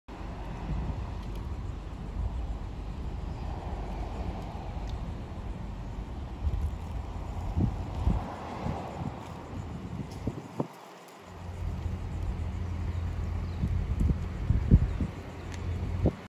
Rode A1A: Mayport Murals
A once drab-looking storage facility in Mayport, Florida, has now been transformed into a collection of stunning wall murals painted by local and national artists as part of a festival hosted by Jacksonville's ArtRepublic. This video shows one of over a dozen wall murals.